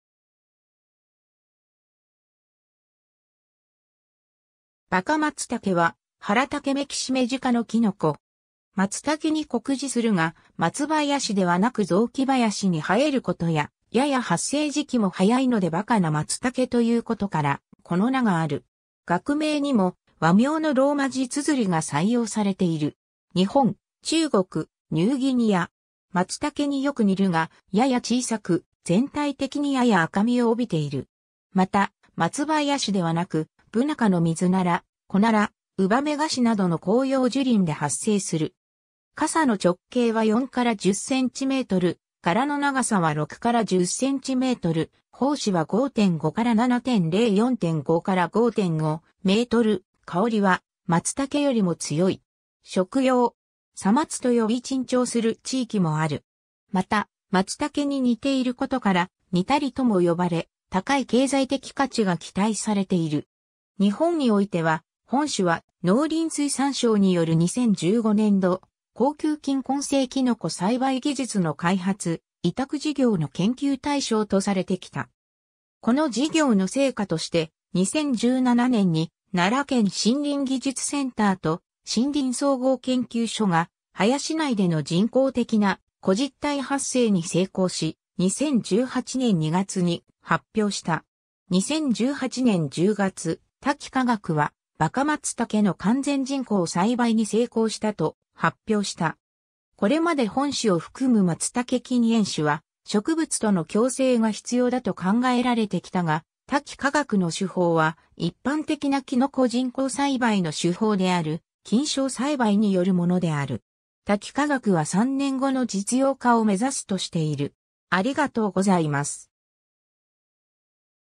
バカマツタケ
バカマツタケ（馬鹿松茸、学名：Tricholoma bakamatsutake）は、ハラタケ目キシメジ科のキノコ。
マツタケに酷似するが、松林ではなく雑木林に生えることや、やや発生時期も早いので馬鹿なマツタケということからこの名がある。
学名にも和名のローマ字綴りが採用されている。
 日本（全土）、中国、ニューギニア。
 マツタケによく似るが、やや小さく全体的にやや赤みを帯びている。
またマツ林ではなく、ブナ科のミズナラ、コナラ、ウバメガシなどの広葉樹林で発生する。
傘の直径は4-10cm、柄の長さは6-10cm、胞子は5.5-7.0×4.5-5.5µm、香りはマツタケよりも強い。
食用。
 「さまつ」と呼び珍重する地域もある。
また、マツタケに似ていることから、｢ニタリ（似たり）｣とも呼ばれ、高い経済的価値が期待されている。
 日本においては本種は、農林水産省による2015年度「高級菌根性きのこ栽培技術の開発」委託事業の研究対象とされてきた。
この事業の成果として、2017年に奈良県森林技術センターと森林総合研究所が林内（屋外）での人工的な子実体発生に成功し、2018年2月に発表した｡ 2018年10月、多木化学はバカマツタケの完全人工栽培に成功したと発表した。
これまで本種を含むマツタケ近縁種は植物との共生が必要だと考えられてきたが、多木化学の手法は一般的なキノコ人工栽培の手法である菌床栽培によるものである。
多木化学は3年後の実用化を目指すとしている。